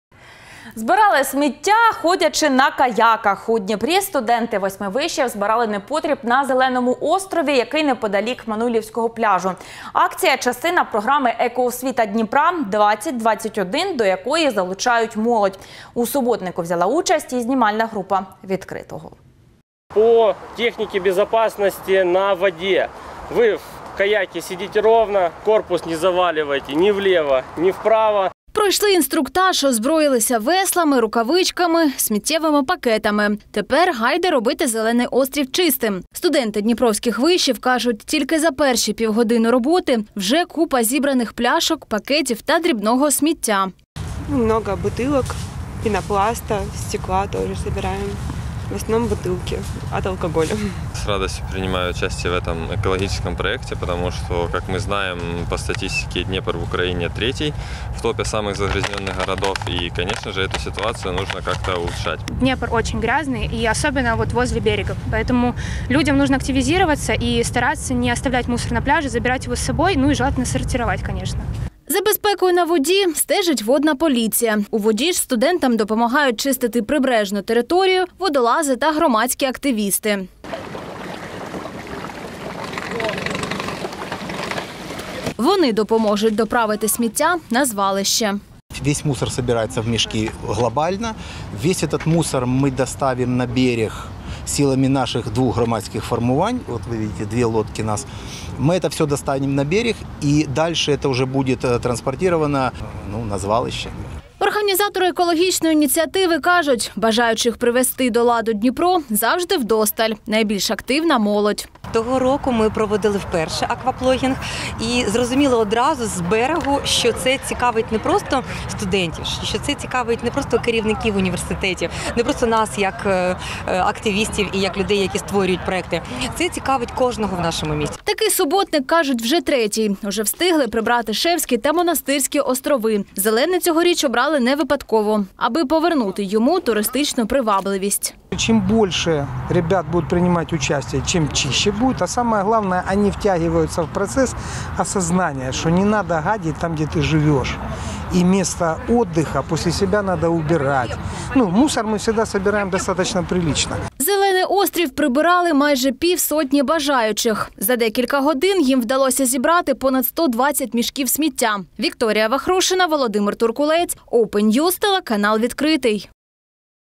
Акваплогінг по-дніпровськи: студенти вишів долучилися до прибирання Зеленого острову
Збирали смiття, ходячи на каяках. У Дніпрі студенти восьми вишів збирали сміття на Зеленому острові, який неподалік Мануйлівського пляжу. Акція – частина програми «Екоосвіта Дніпра-2021», до якої залучають молодь.